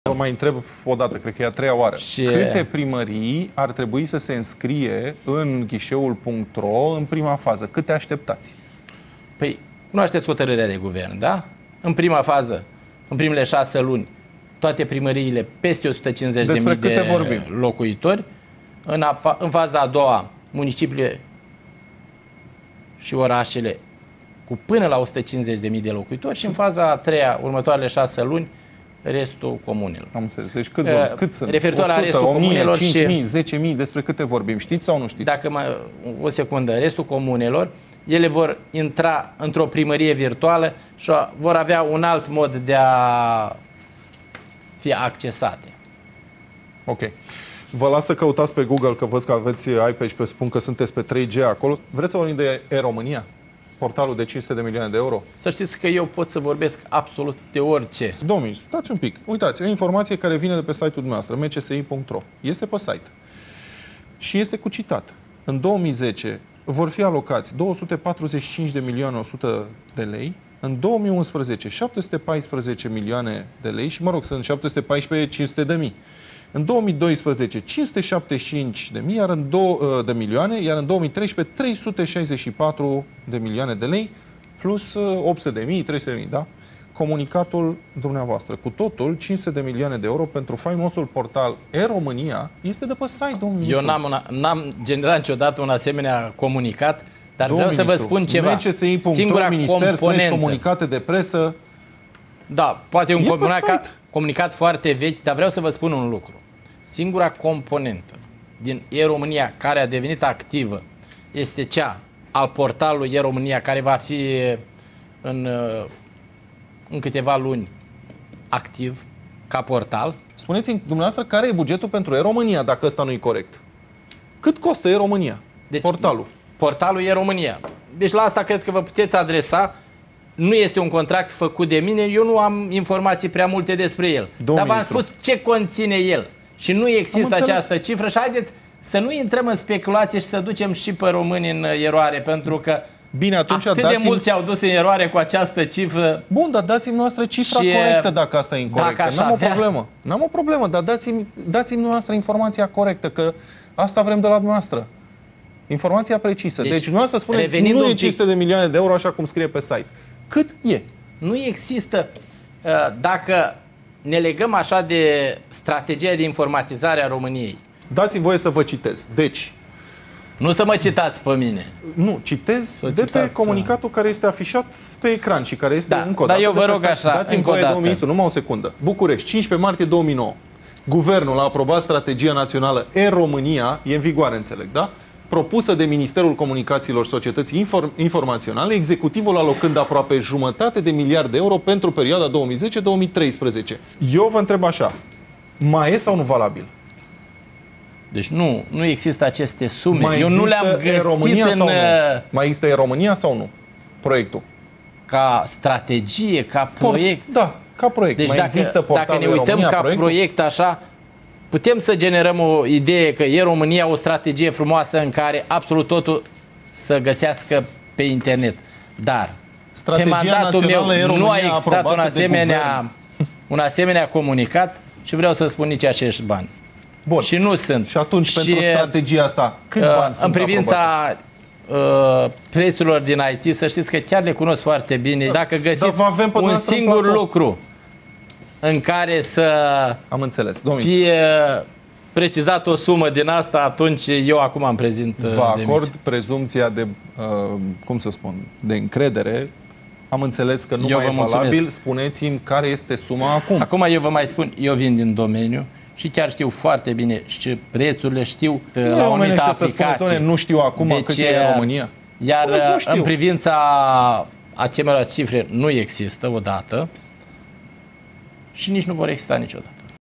Ministrul Comunicatiilor nu stie mai nimic
Ministrul Comunicatiilor, Valerian Vreme (PDL) intervievat la X-Press (iulie 2010), nu reuseste sa raspunda la inrebari simple care se refera strict la ceea ce ar trebui sa coordoneze (ghiseul.ro, e-Romania)